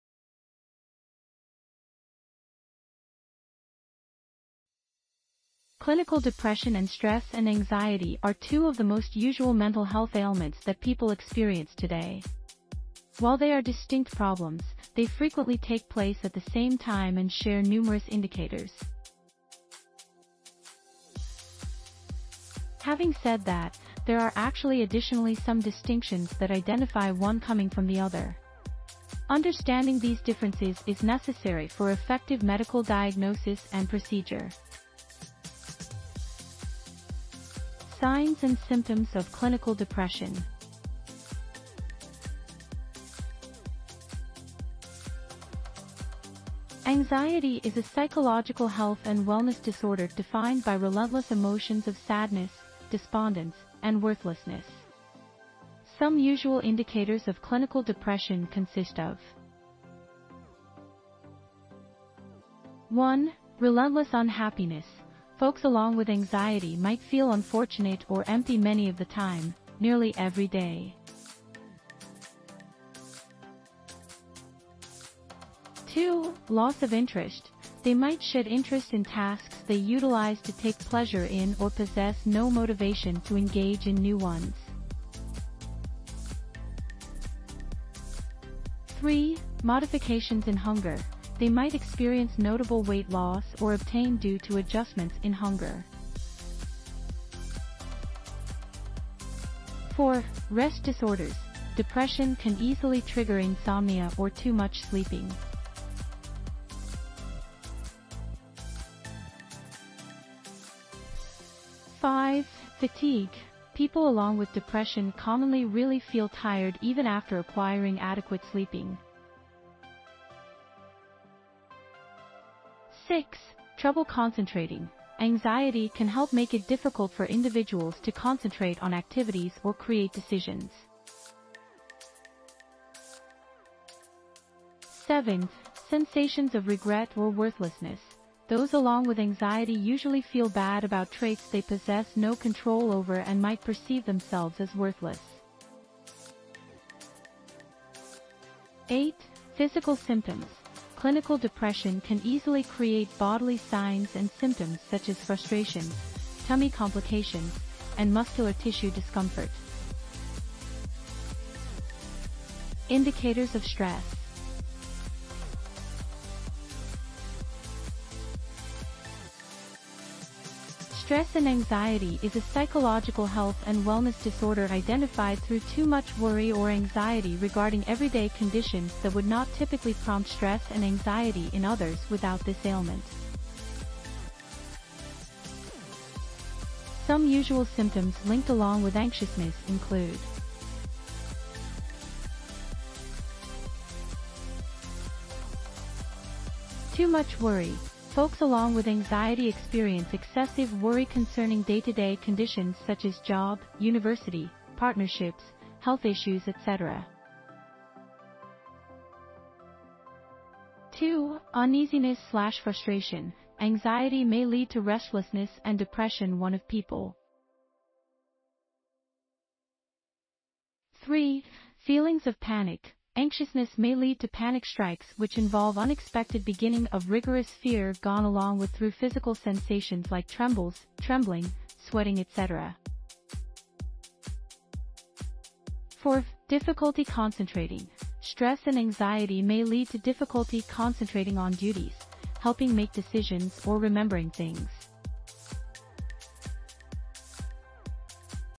Facts About "What Sets Depression Apart from Anxiety?" Revealed
Clinical depression and stress and anxiety are two of the most usual mental health ailments that people experience today. While they are distinct problems, they frequently take place at the same time and share numerous indicators. Having said that, there are actually additionally some distinctions that identify one coming from the other. Understanding these differences is necessary for effective medical diagnosis and procedure.

Signs and symptoms of Clinical depression

Anxiety is a psychological health and wellness disorder defined by relentless emotions of sadness, despondence, and worthlessness. Some usual indicators of clinical depression consist of:

1. Relentless unhappiness: Folks along with anxiety might feel unfortunate or empty many of the time, nearly every day.

2. Loss of interest: They might shed interest in tasks they utilized to take pleasure in or possess no motivation to engage in new ones.

3. Modifications in hunger: They might experience notable weight loss or obtain due to adjustments in hunger.

4. Rest disorders: Depression can easily trigger insomnia or too much sleeping.

5. Fatigue: People along with depression commonly really feel tired even after acquiring adequate sleeping.

6. Trouble concentrating: Anxiety can help make it difficult for individuals to concentrate on activities or create decisions.

7. Sensations of regret or worthlessness: Those along with anxiety usually feel bad about traits they possess no control over and might perceive themselves as worthless.

8. Physical symptoms: Clinical depression can easily create bodily signs and symptoms such as frustrations, tummy complications, and muscular tissue discomfort.

Indicators of Stress

Stress and anxiety is a psychological health and wellness disorder identified through too much worry or anxiety regarding everyday conditions that would not typically prompt stress and anxiety in others without this ailment.

Some usual symptoms linked along with anxiousness include:

1. Too much worry: Folks along with anxiety experience excessive worry concerning day-to-day conditions such as job, university, partnerships, health issues etc

2. Uneasyness/Frustration : Anxiety may lead to restlessness and depression one of people

3.Feelings Of Panic : Anxiousness may lead to panic strikes which involve unexpected beginning of rigorous fear gone along with through physical sensations like trembles , trembling , sweating etc

4.Difficulty Concentrating: Stress and anxiety may lead to difficulty concentrating on duties, helping make decisions or remembering things

5.Sleep disturbances: Stress may result in difficulty dropping asleep, remaining asleep or experiencing problems

6.Physical Signs: People along with stress may experience bodily symptoms such as sweating, trembling, pulsations, and shortness of breathing.

7.Avoidance behaviour: Individuals along with anxiety make an effort to avoid situations or spot that can induce their anxiousness.

8. Fanatical thought and feelings: Stress and anxiety may lead to obsessive thoughts concerning specific factors like hygiene , safety etc.

How are clinical depression and stress and anxiety similar?

Depression and anxiousness discuss numerous signs including rest disturbances, problem concentrating, bodily signs and symptoms such as problems and stomach complications. Each problems likewise possess a unfavorable impact on an individual's high quality of life by meddling along with work/school efficiency, partnerships and total well-being. They are additionally usually comorbid - implying that folks who suffer coming from one problem are at much higher risk for cultivating the various other.

How are clinical depression and stress various?

While there is actually overlap in the signs and symptoms of depression and anxiousness , there are some crucial distinctions between the two ailments.

One major difference is the emphasis of the mental condition . Individuals along with depression usually tend to feel unfortunate or desperate while those with anxiety often tend to experience afraid  which might show up in means like extreme worry , restlessness etc .

Another difference lies in their effect on regular functioning . Anxiety frequently creates a absence of enthusiasm in tasks while individuals suffering coming from stress may steer clear of tas...